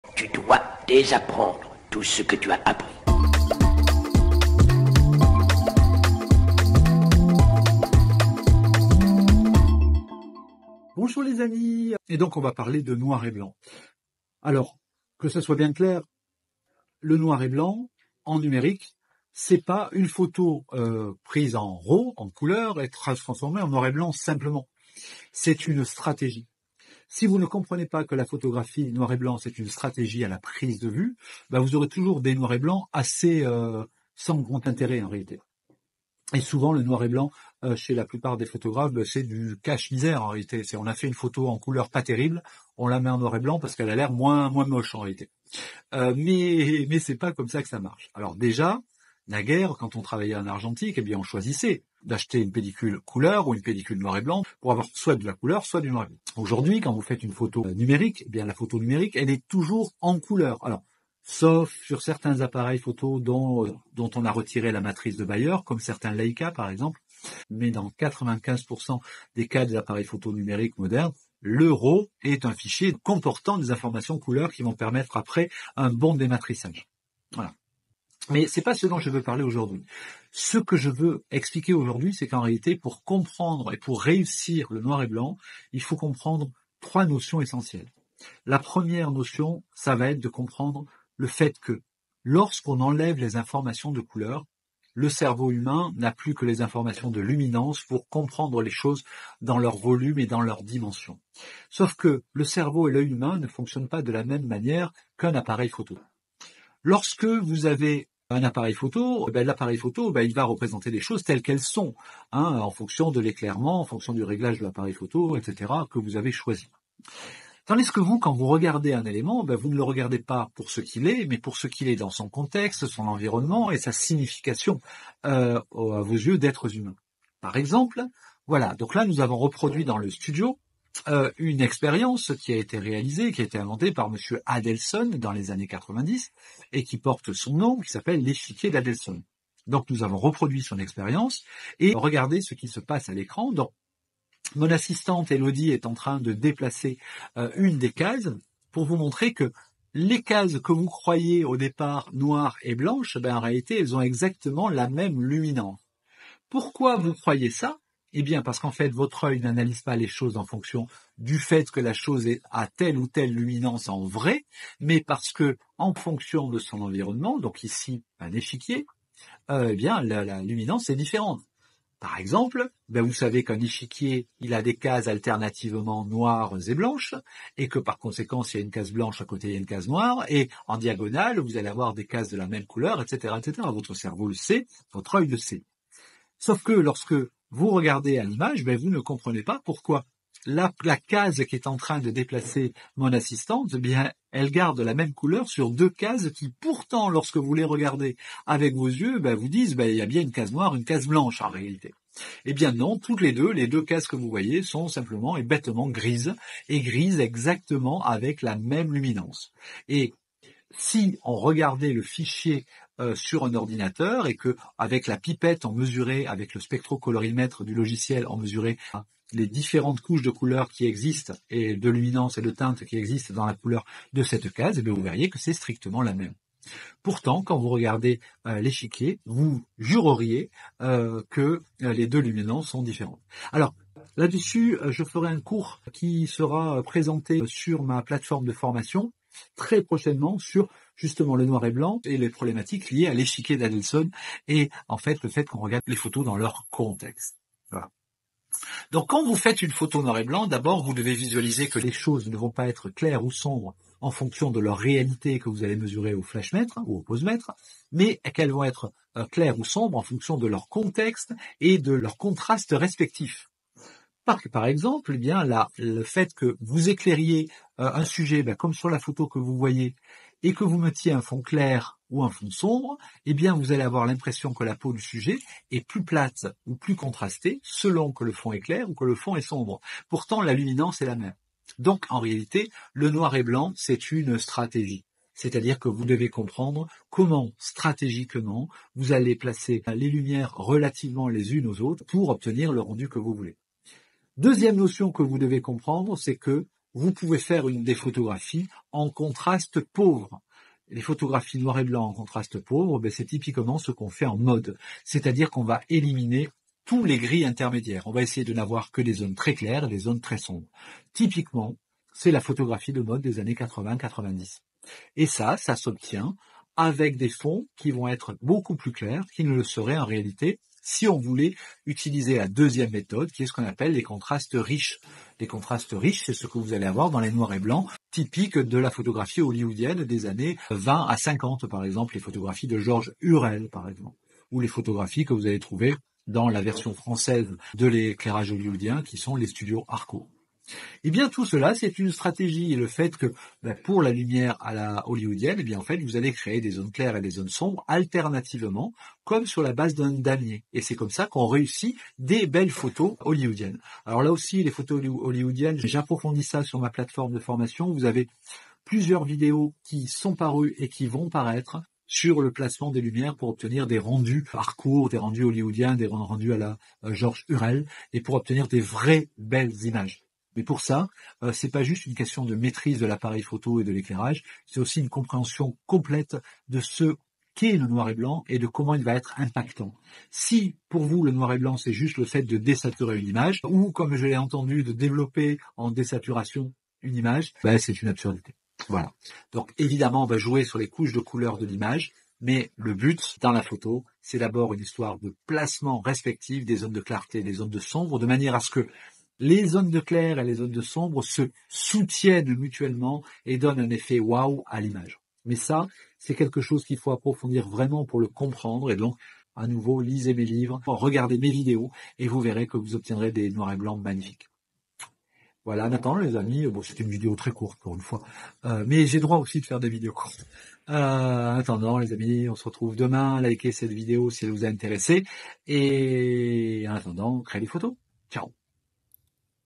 126 - La photo noir est blanc est une stratégie
A la suite de l'expérience publiée hier (NSF n°125), j'explique la stratégie qui permet d'avoir des noirs et blancs sublimes...